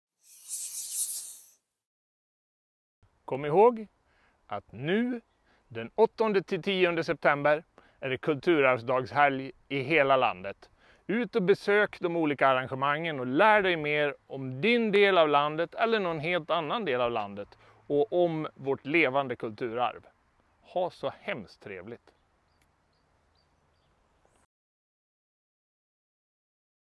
Välkommen till Kulturarvsdagen 2023!
Årets tema: LEVANDE KULTURARV

Kulturarvsdagen är Riksantikvarieämbetets årliga evenemang där lokala arrangörer medverkar och bjuder in allmänheten till hundratals olika kulturarvsarrangemang från norr till söder i hela landet.

Den nationella temadagen för kulturarvet har arrangerats sedan 1988 med olika teman varje år. Kulturarvsdagen infaller i år söndagen den 10 september.  Som vanligt kan man medverka med eller delta i aktiviteter även redan under fredagen och lördagen, den 8 och 9 september, så att så många besökare som möjligt får tillfälle att uppleva kulturarvet – på plats eller genom digitala aktiviteter.